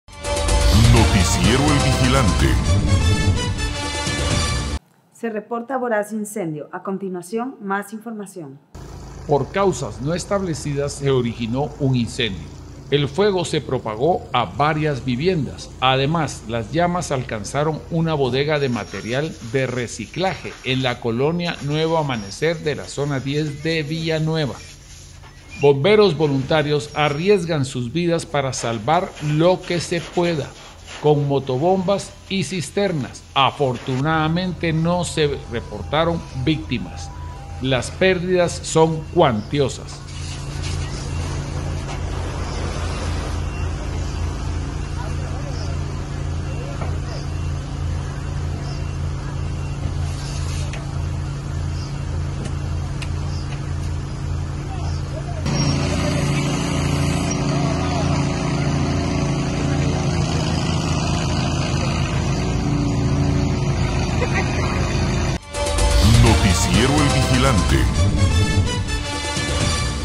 Incendio arrasó con varias viviendas en Villa Nueva
Por causas no establecidas se originó un incendio, el fuego se propagó a varias viviendas, además las llamas alcanzaron una bodega de material de reciclaje en la Colonia Nuevo Amanecer zona 10 de Villa Nueva